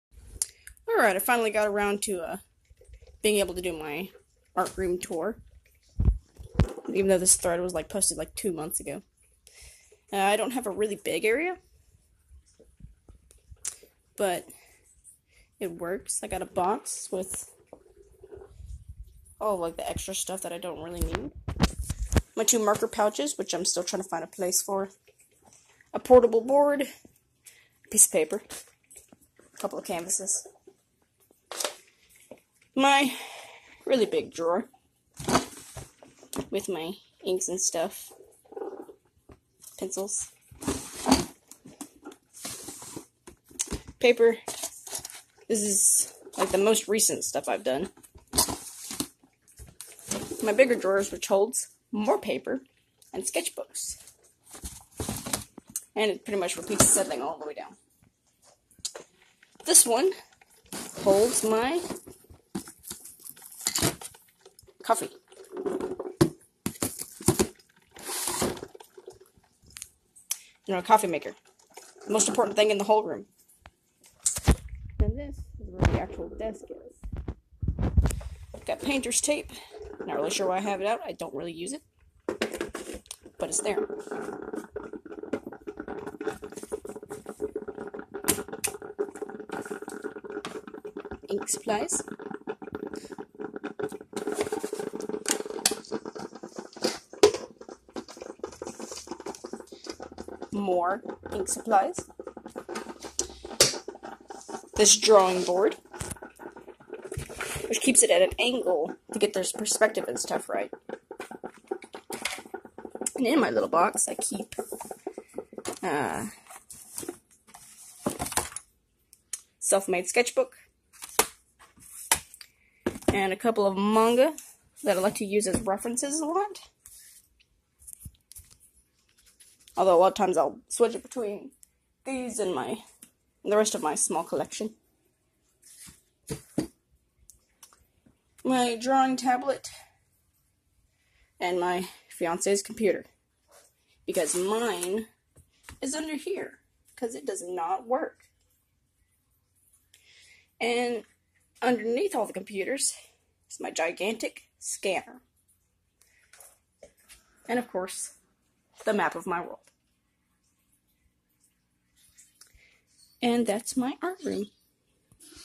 Art Room Tour!
Sorry for the lack of uploads. My computer is DEAD, and thus I cannot record.
I use my fiance's computer for references, research, and entertainment, but I do not want to bombard his poor machine with heavy programs that I use. SO. Until I get a new computer, this phone video will have to do!